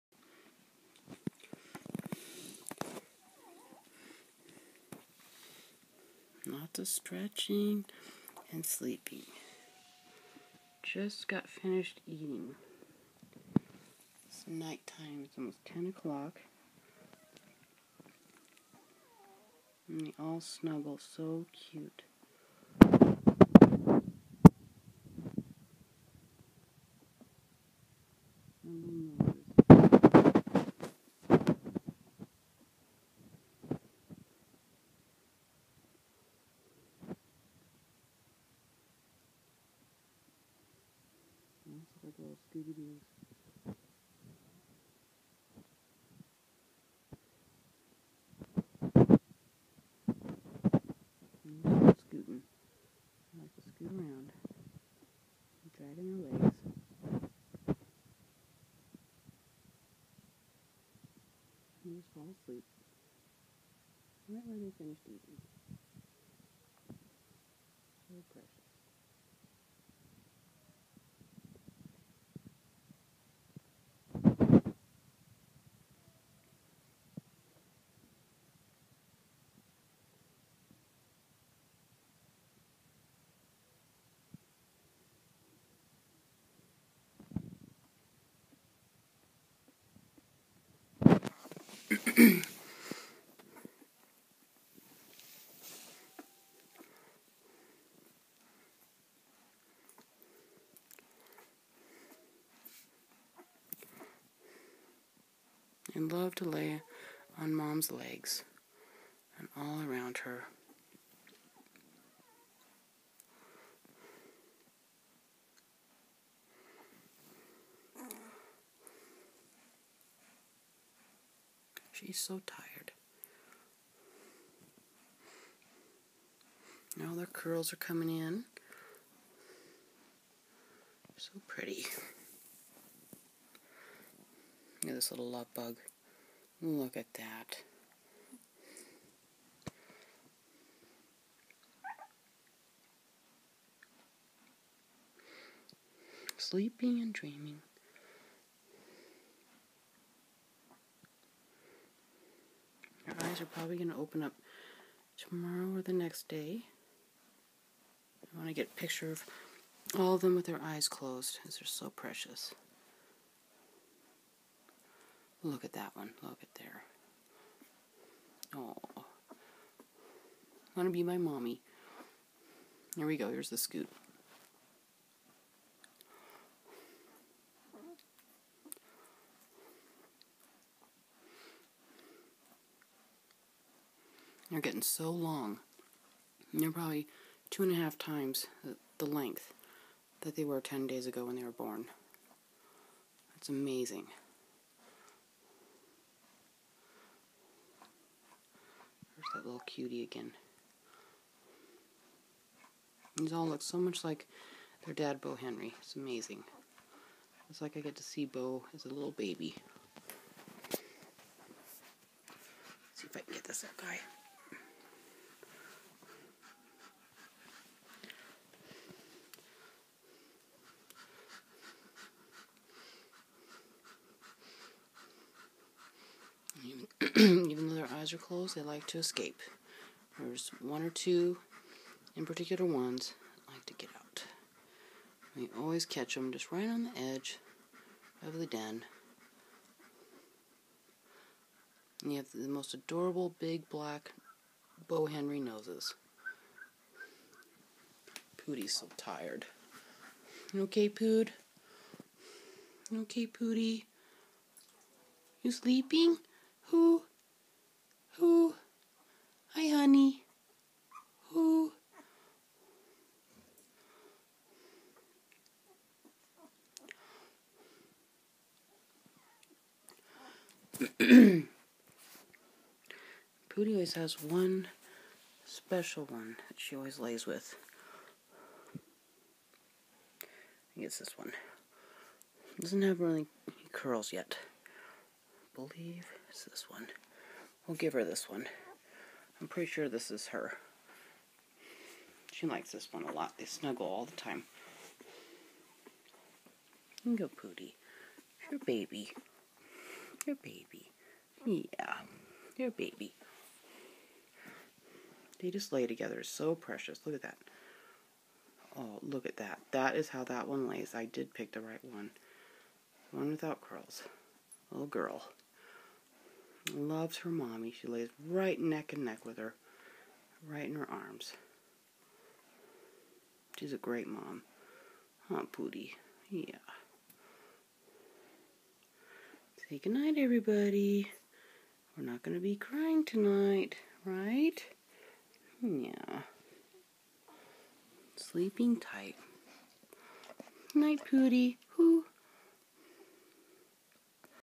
Sweetheartdoodles
A short clip of Poodie Bear's puppies filmed before their eyes opened.